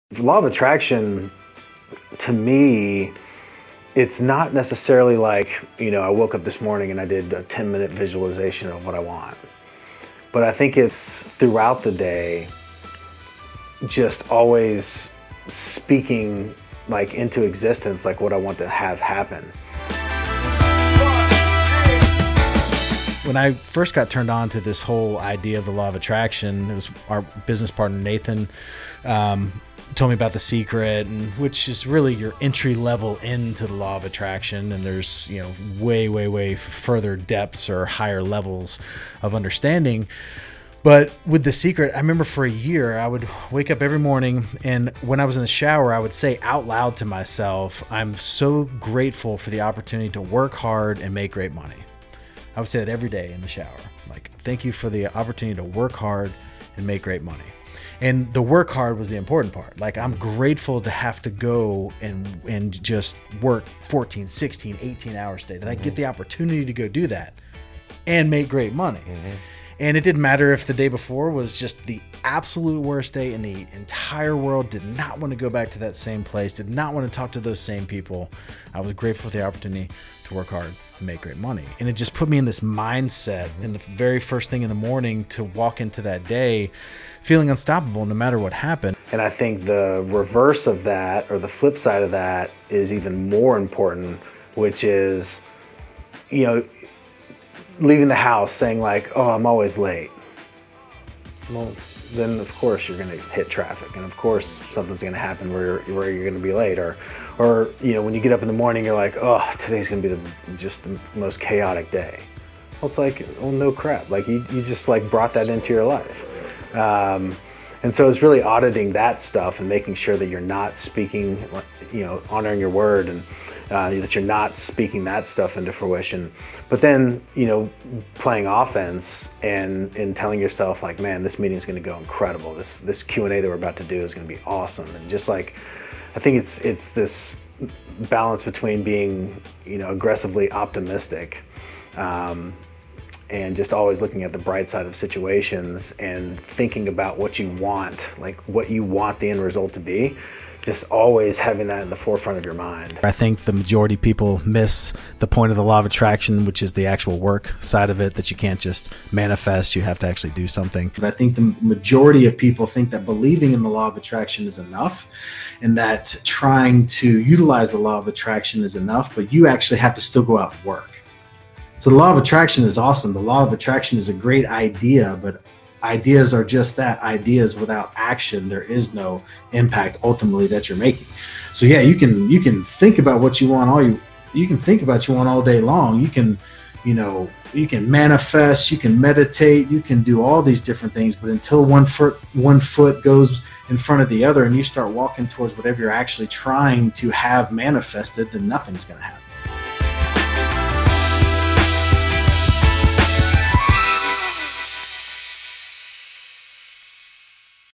Road To Legacy | Episode 264
The Law of Attraction isn't something that mystical and out there. It's just speaking positivity into your life and putting you in the best mindset to enter your day with.